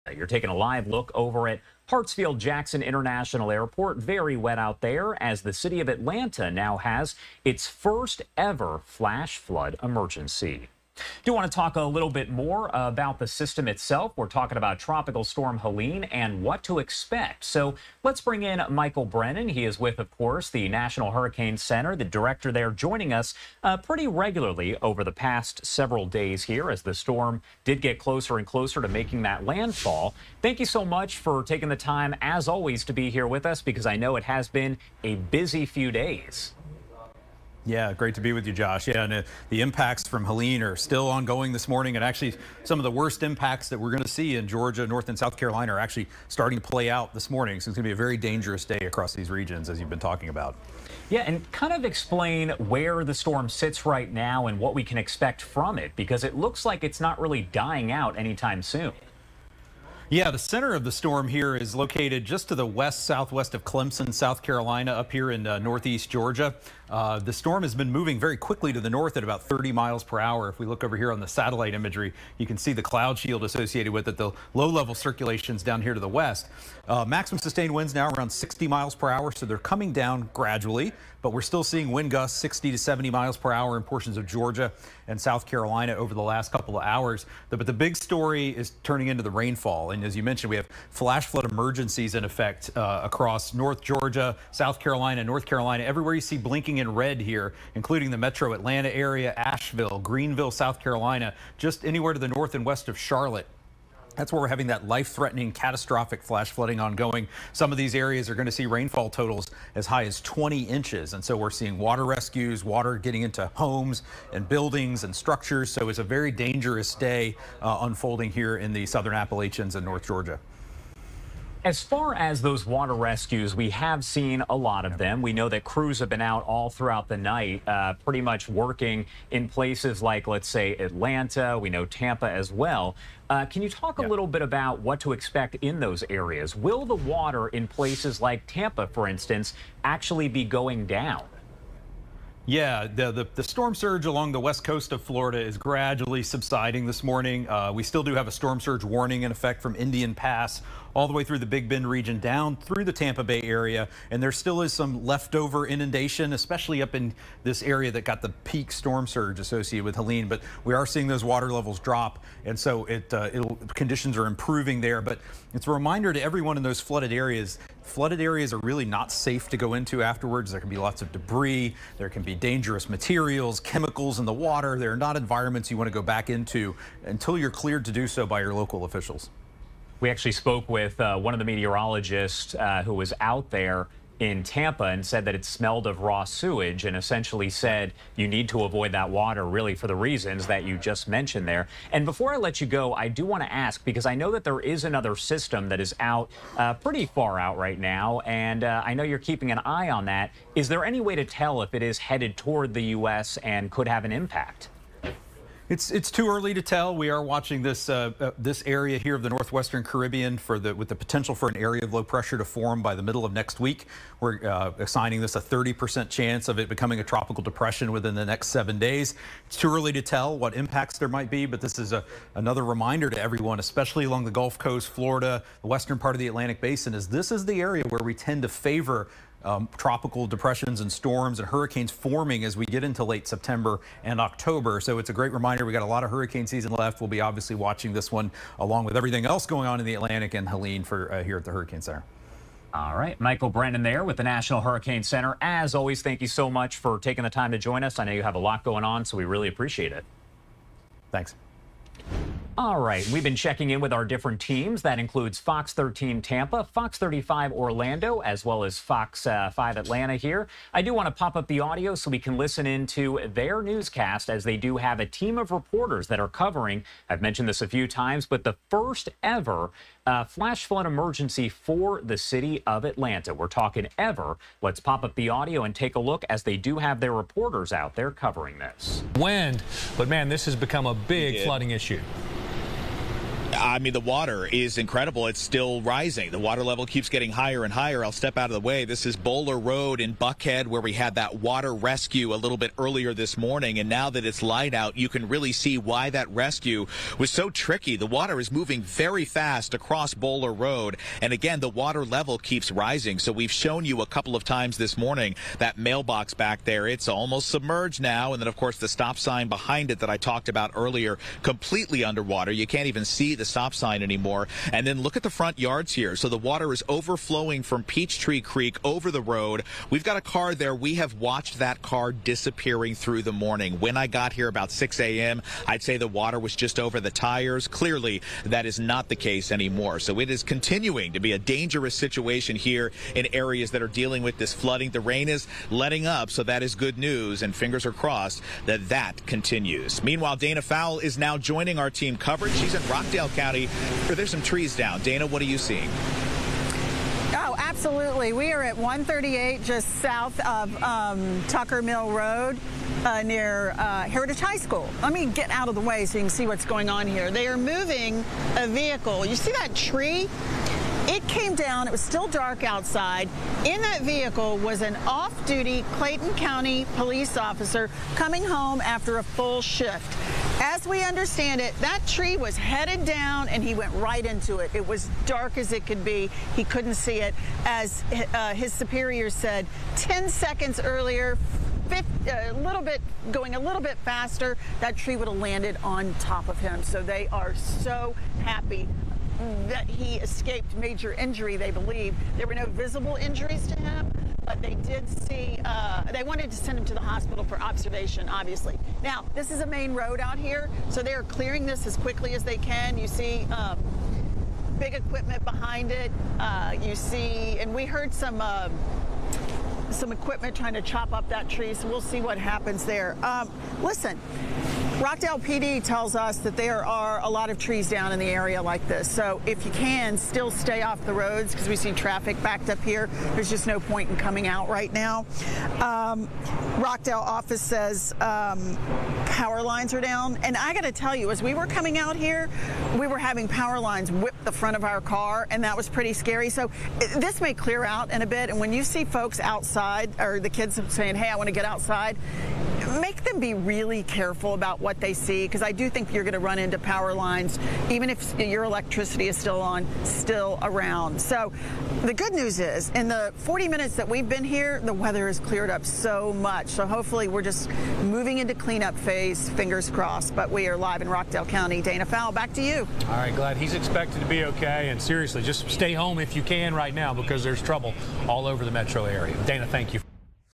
Atlanta sees first-ever flash flood emergency with multiple water rescues | LiveNOW from FOX
Atlanta was placed under its first-ever flash flood emergency on Friday morning as the city endures its wettest three-day period in 104 years. The National Hurricane Center and FOX 5 Atlanta discussed the latest developments.